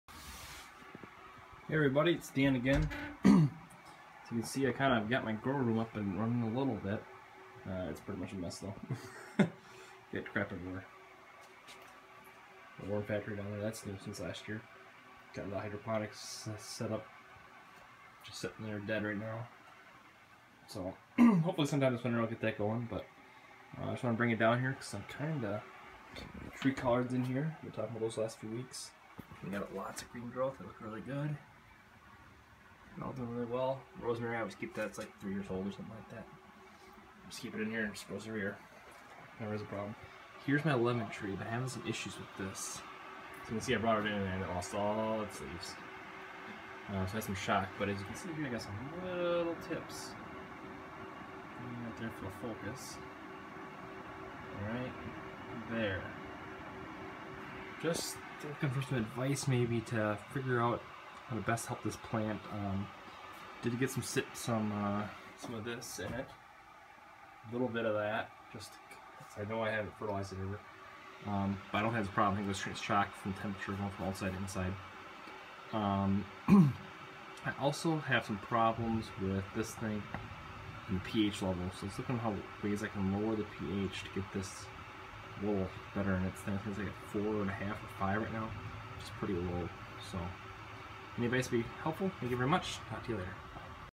Poor lemon tree, grow room in shambles
Next time I will do the video in the correct aspect ratio ha ha